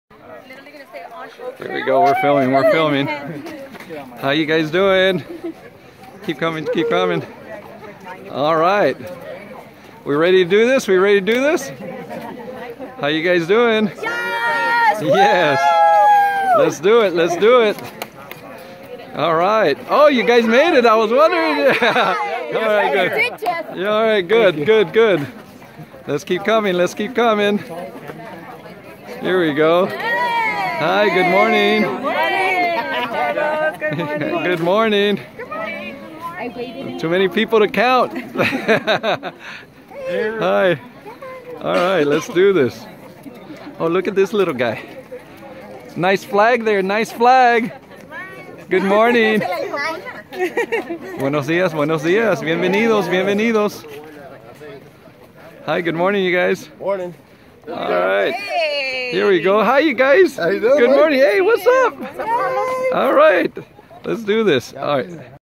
September 11th Memorial Hike to Top of The World in Laguna
For the 16th anniversary of September11 we decided to host a memorial hike to Top of The World in Laguna.
This was a collaboration hike between
@Janet_1029 , @ResilientSquad & @EveryonesHike (Instagram names).

September11MemorialHike to honor those lost due to the events that took place on the morning of September11th, 2001.

Our color theme was RED, WHITE and BLUE. 🇺🇸 It was an awesome hike with an amazing group of hikers.  Everyone was friendly, bonded, helped and encouraged each other.  Oh and there were plenty of photos taken,  lol.
We are so blessed to be able to do what we do.
Thank you to all of those who joined us on this memorial hike.